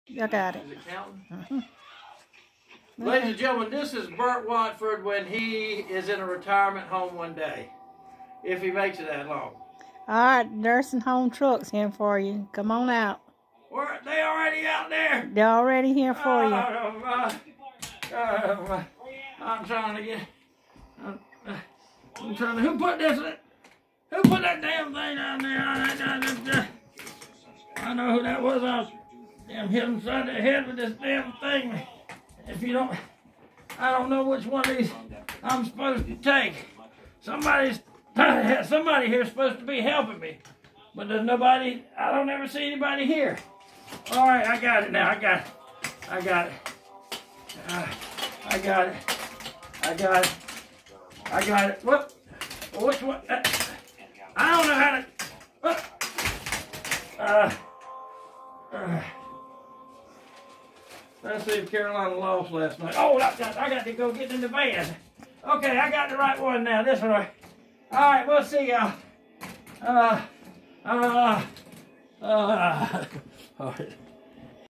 Nursing home
Wonder how Bert will handle being there ?